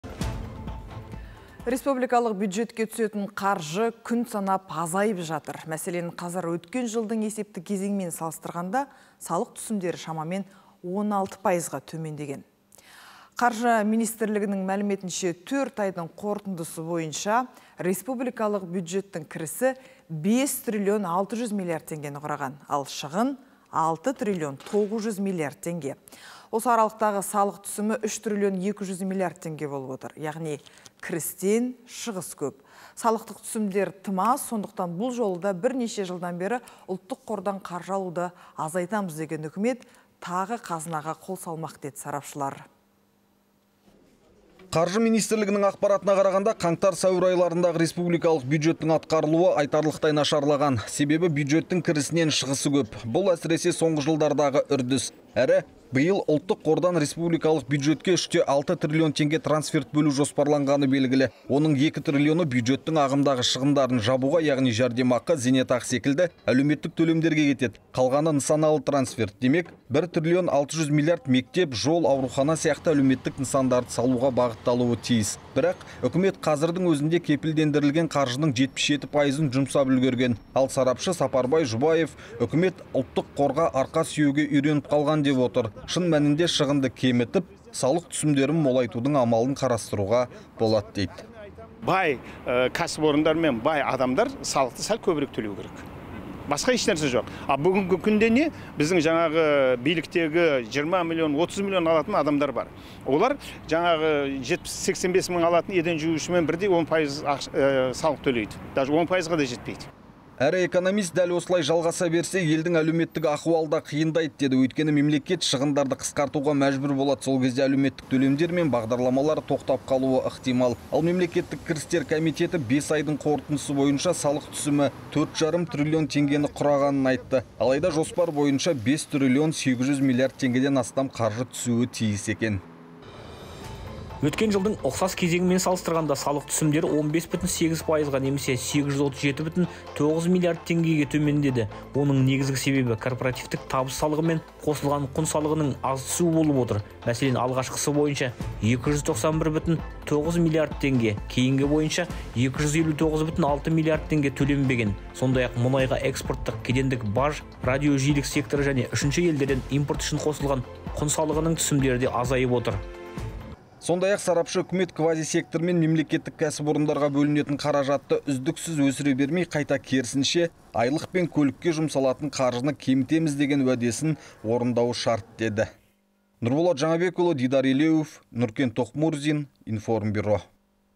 Байлар аз төлей ме? Республикалық бюджетке түсетін салық 16 пайызға төмендеп кетті
Республикалық бюджетке түсетін қаржы күн санап азайып жатыр. Мәселен, қазір өткен жылдың есепті кезеңімен салыстырғанда салық түсімдері шамамен 16 пайызға төмендеген. Қаржы министрлігінің мәліметінше, төрт айдың қорытындысы бойынша республикалық бюджеттің кірісі 5 триллион 600 миллиард теңгені құраған. Ал, шығын 6 триллион 900 миллиард теңге. Осы аралықтағы салық түсімі 3 триллион 200 миллиард теңге болып отыр. Яғни, кірістен шығыс көп. Салықтық түсімдер тым аз. Сондықтан, бұл жолы да бірнеше жылдан бері Ұлттық қордан қаржы алуды азайтамыз деген Үкімет тағы қазынаға қол салмақ деді сарапшылар

Бүгінгі шығарылымда:

Қаржы министрлігінің ақпаратына қарағанда, қаңтар-сәуір айларындағы республикалық бюджеттің атқарылуы айтарлықтай нашарлаған. Себебі, бюджеттің кірісінен шығысы көп. Бұл әсіресе соңғы жылдардағы үрдіс. Әрі, биыл Ұлттық қордан республикалық бюджетке 3,6 триллион теңге трансферт бөлу жоспарланғаны белгілі. Оның 2 триллионы бюджеттің ағымдағы шығындарын жабуға, яғни жәрдемақы, зейнетақы секілді әлеуметтік төлемдерге кетеді. Қалғаны нысалы трансферт. Демек, 1 триллион 600 миллиард мектеп, жол, аурухана сияқты әлеуметтік нысандарды салуға бағытталуы тиіс. Бірақ Үкімет қазірдің өзінде кепілдендірілген қаржының 77 пайызын жұмсап үлгерген. Ал сарапшы Сапарбай Жұбаев Үкімет Ұлттық қорға арқа сүйеуге үйреніп қалған деп отыр. Шын мәнінде шығынды кемітіп, салық түсімдерін молайтатудың амалын қарастыруға болады дейді.
САПАРБАЙ ЖҰБАЕВ, экономист:
– Бай кәсіпорындар мен бай адамдар салықты сәл көбірек төлеуі керек. Басқа ешнәрсе жоқ. Ал бүгінгі күнде не? Біздің жаңағы биліктегі 20 миллион, 30 миллион алатын адамдар бар. Олар жаңағы 70-85 мың алатын еден жуушымен бірдей 10 пайыз салық төлейді. Даже 10 пайызға да жетпейді.
Әрі экономист дәл осылай жалғаса берсе, елдің әлеуметтік ахуалы да қиындайды деді. Өйткені, мемлекет шығындарды қысқартуға мәжбүр болады. Сол кезде әлеуметтік төлемдер мен бағдарламалар тоқтап қалуы ықтимал. Ал мемлекеттік кірістер комитеті 5 айдың қорытындысы бойынша салық түсімі 4,5 триллион теңгені құрағанын айтты. Алайда жоспар бойынша 5 триллион 800 миллиард теңгеден астам қаржы түсуі тиіс екен.
ҚР МЕМЛЕКЕТТІК КІРІСТЕР КОМИТЕТІНІҢ БАСПАСӨЗ ҚЫЗМЕТІ:
– Өткен жылдың ұқсас кезеңімен салыстырғанда салық түсімдері 15,8%-ға немесе 837,9 миллиард теңгеге төмендеді. Оның негізгі себебі корпоративтік табыс салығы мен қосылған құн салығының аз түсуі болып отыр. Мәселен, алғашқысы бойынша 291,9 миллиард теңге, кейінгі бойынша 259,6 миллиард теңге төленбеген. Сондай-ақ, мұнайға экспорттық кедендік баж, радиожиілік секторы және үшінші елдерден импорт үшін қосылған құн салығының түсімдері де азайып отыр.
Сондай-ақ, сарапшы Үкімет квазисектор мен мемлекеттік кәсіпорындарға бөлінетін қаражатты үздіксіз өсіре бермей, қайта керісінше айлық пен көлікке жұмсалатын қаржыны кемітеміз деген уәдесін орындауы шарт деді.

Біз барлық әлеуметтік желідеміз, тіркеліңіз:
Бізді әлеуметтік желіде оқыңыздар: